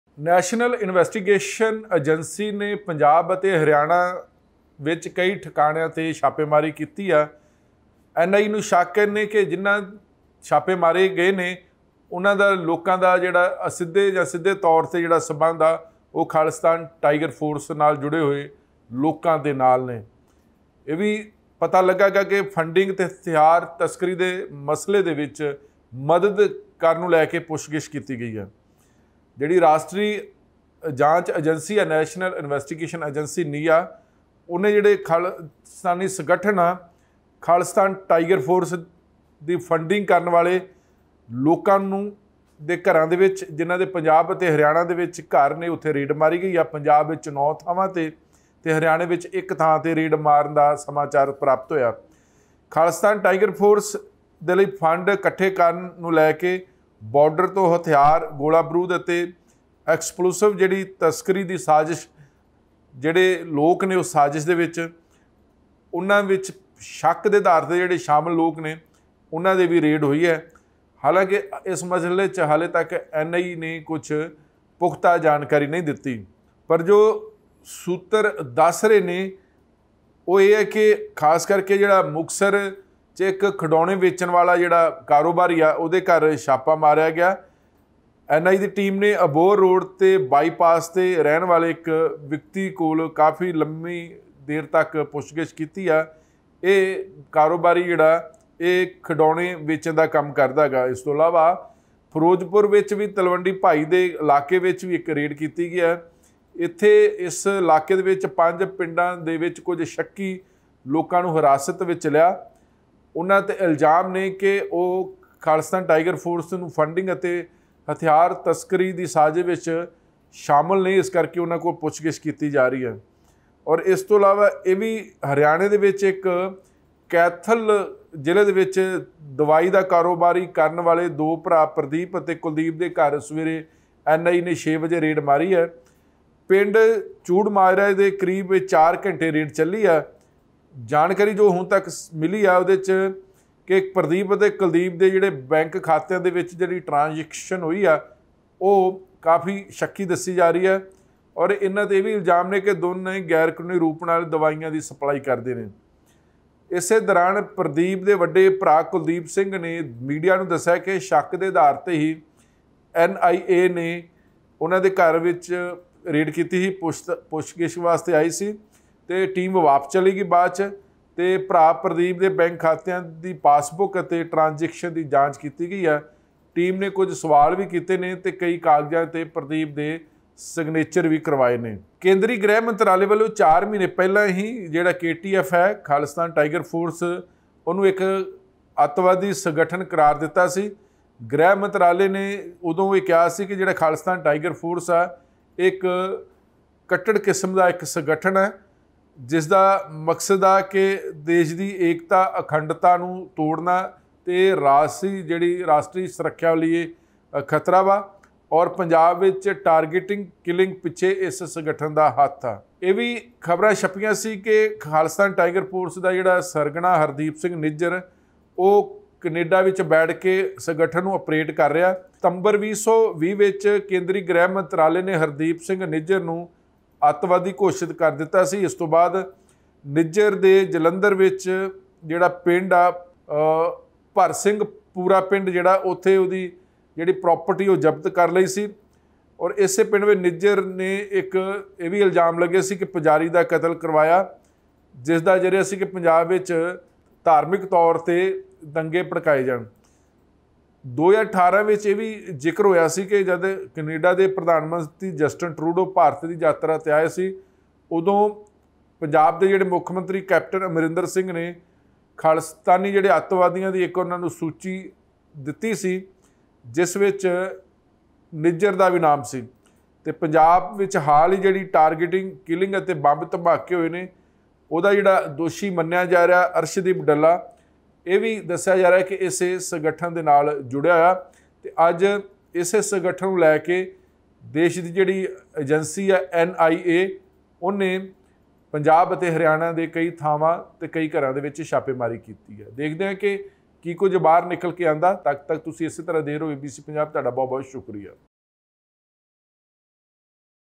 NIA ਦੀ Arm ਡੀਲਰ ਤੇ ਵੱਡੇ ਕਾਰੋਬਾਰੀ ਦੇ ਘਰਾਂ 'ਚ ਛਾਪੇ, ਤਾਰਾਂ ਵਿਦੇਸ਼ਾਂ 'ਚ ਜੁੜੀਆਂ
NIA ਦੀ Arm ਡੀਲਰ ਤੇ ਵੱਡੇ ਕਾਰੋਬਾਰੀ ਦੇ ਘਰਾਂ 'ਚ ਛਾਪੇ
ਤਾਰਾਂ ਵਿਦੇਸ਼ਾਂ 'ਚ ਜੁੜੀਆਂ